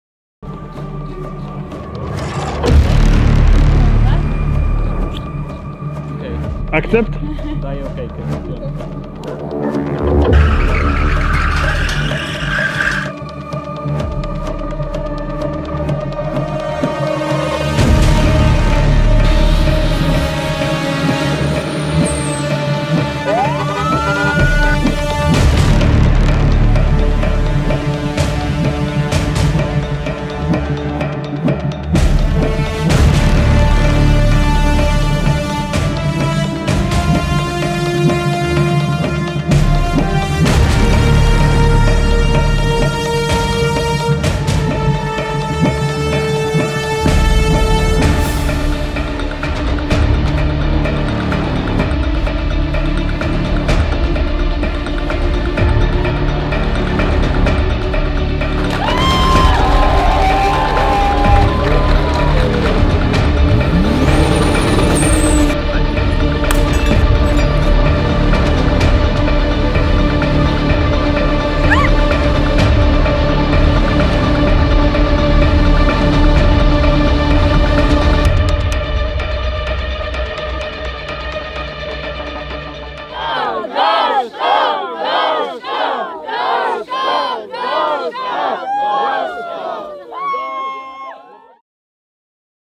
BMW Lover | Victoria i Kacper | Hotel na Półboru | Artystyczny skrót ślubu
❤️🤭Gradacja emocji, szalona impreza weselna, mnóstwo miłości i szczęścia! Niesamowite dwa dni spędzone z Victorią i Kacprem oraz wspaniałymi gośćmi!🎥 Dziękujemy!!! 📸PS Wciąż czujemy zapach palonych opon BMW😅 SZALEŃCY!🔥🚔 Koniecznie dajcie znać jak Wam się podoba? Bo my już wiemy, że Naszej Parze i ich rodzicom bardzoooo!😍
Nasza pasja daje Nam niesamowitą siłę do działania i tworzenia dla Was przepięknych wspomnień na zawsze!🫶